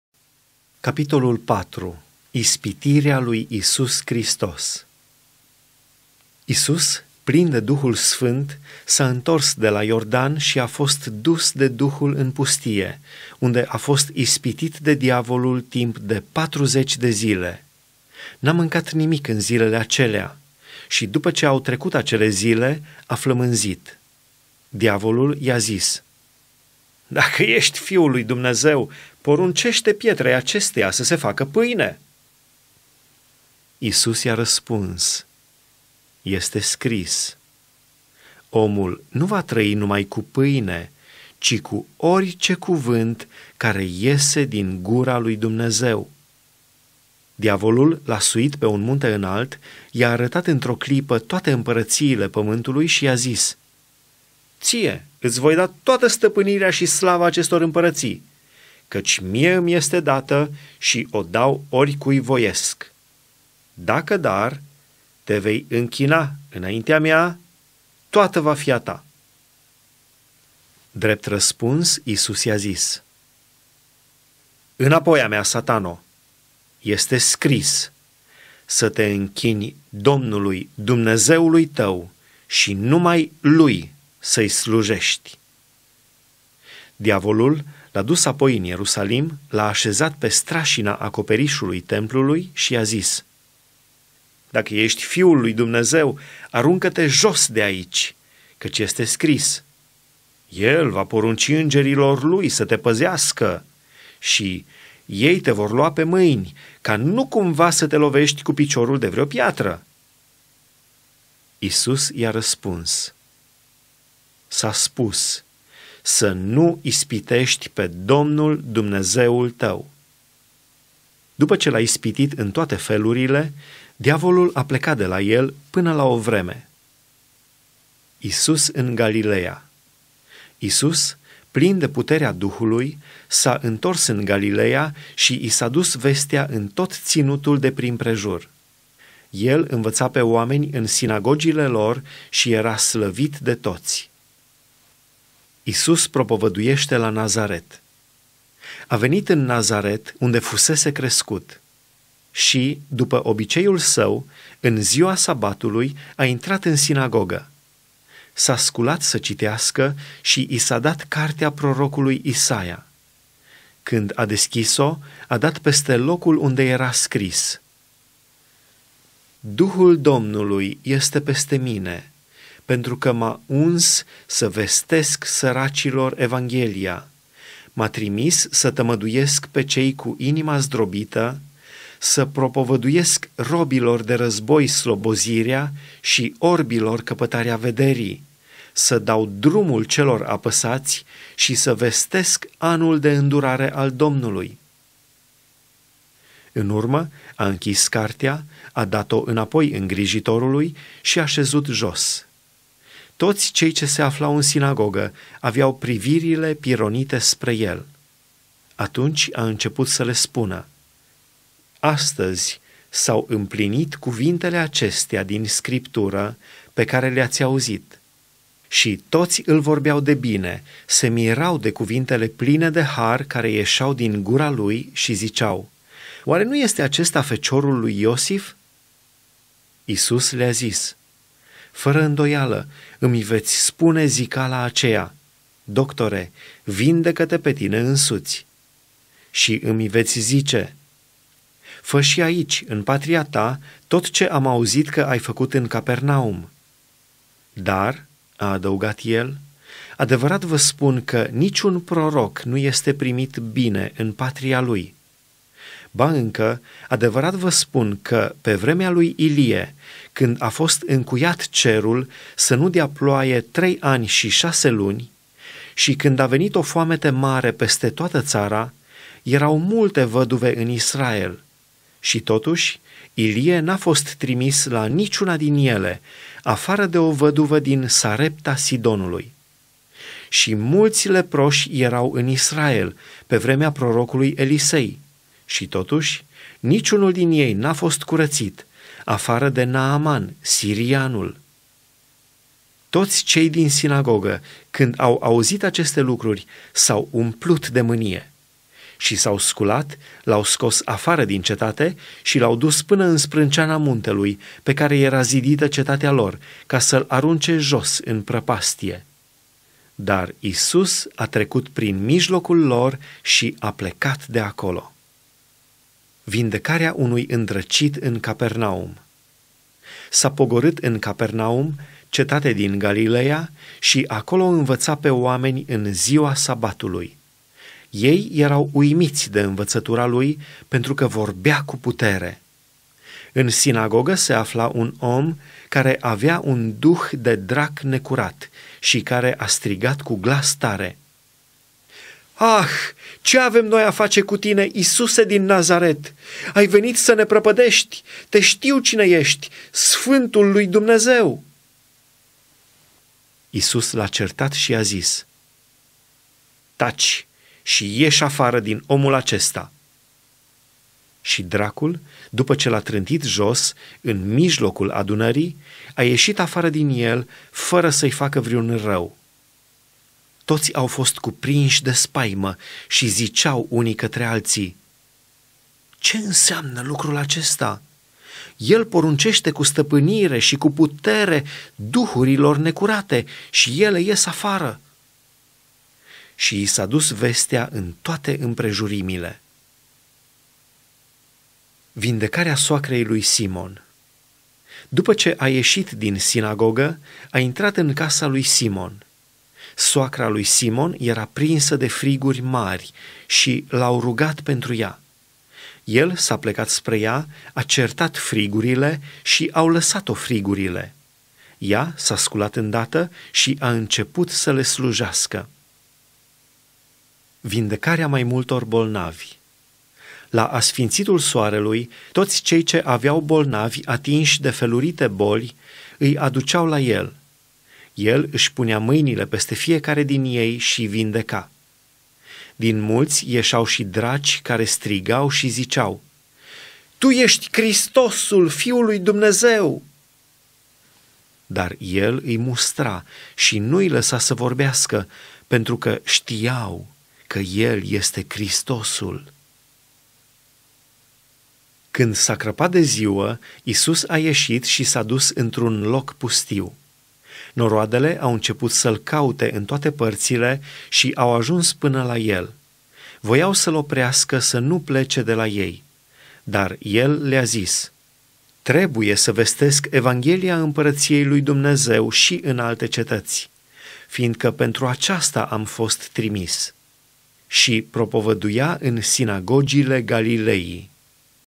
LUCA 4 - Ispitirea lui Isus Hristos
00:00:00   Ispitirea lui Isus Hristos
00:02:07  Isus în Galileea
00:02:25  Isus propovăduieşte la Nazaret
00:05:15  Vindecarea unui îndrăcit în Capernaum
00:06:31  Vindecarea soacrei lui Simon
00:06:56  Vindecarea mai multor bolnavi